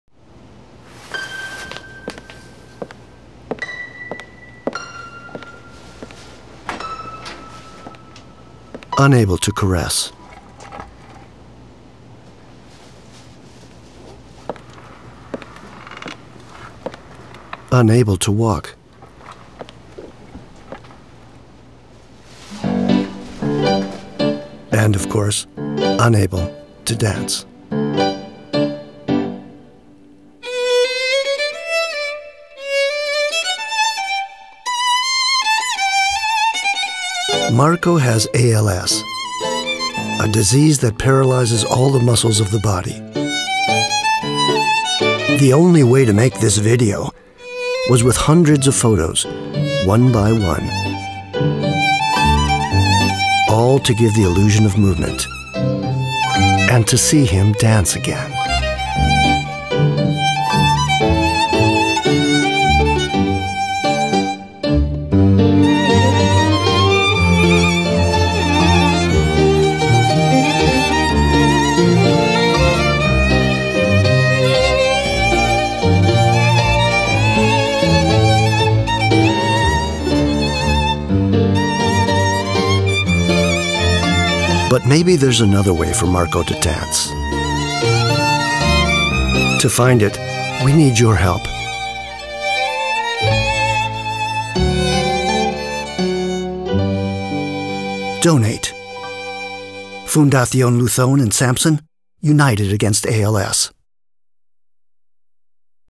Start the Dance: a dance from Luzón Foundation and Samsung.
Marco has ALS: the disease that paralyzes even the last of your muscles. Photo by photo, through the stop motion technique and the technology of the new Samsung Galaxy S9 +, we have created the illusion of movement in Marco's body.
Help us to make this dance come true with more research and assistance.

Dancers: Marco Sánchez Becerra and Alejandra Capello
Choreography: Alejandra Capello
Director: Julián Zuazo